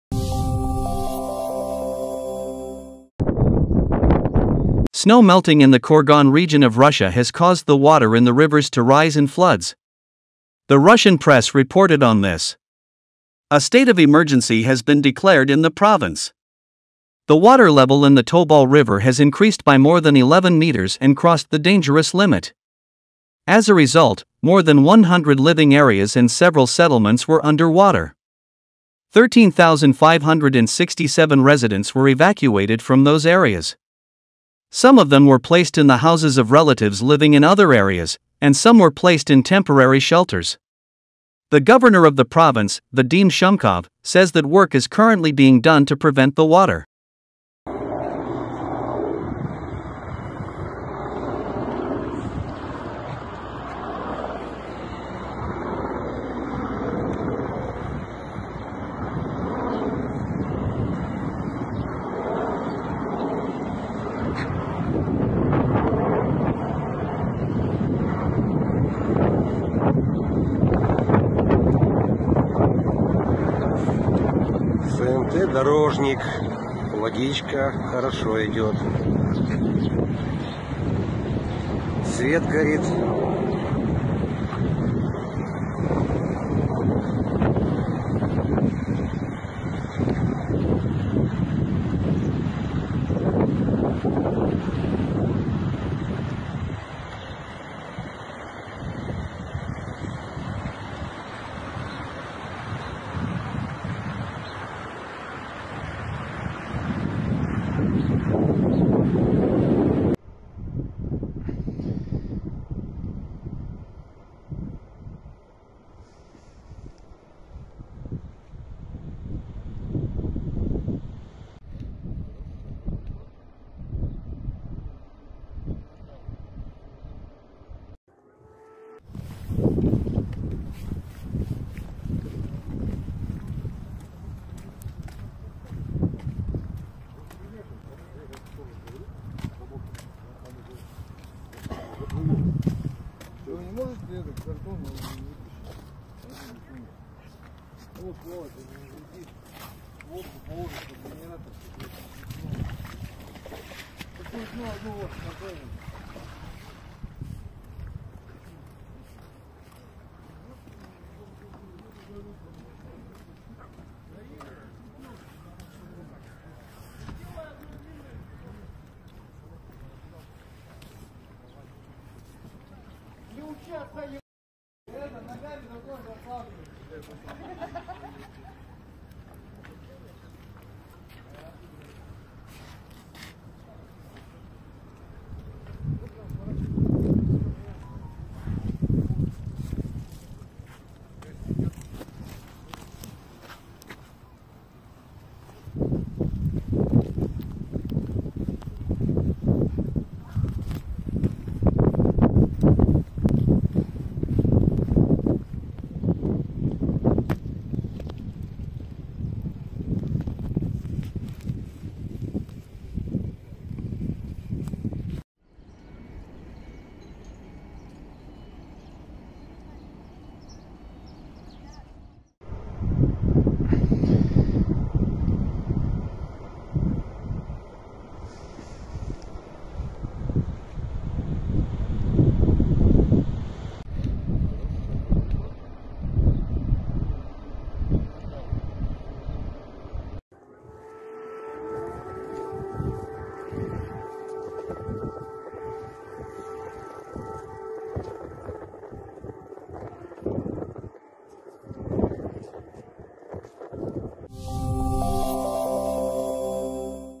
Water level in Russia’s Kurgan region reaches critical level - Thousands of people are evacuated
Snow melting in the Kurgan region of Russia has caused the water in the rivers to rise and floods. The Russian press reported on this. A state of emergency has been declared in the province. The water level in the Tobol River has increased by more than 11 meters and crossed the dangerous limit. As a result, more than 100 living areas and several settlements were under water. 13,567 residents were evacuated from those areas. Some of them were placed in the houses of relatives living in other areas, and some were placed in temporary shelters. The governor of the province, Vadim Shumkov, says that work is currently being done to prevent the water.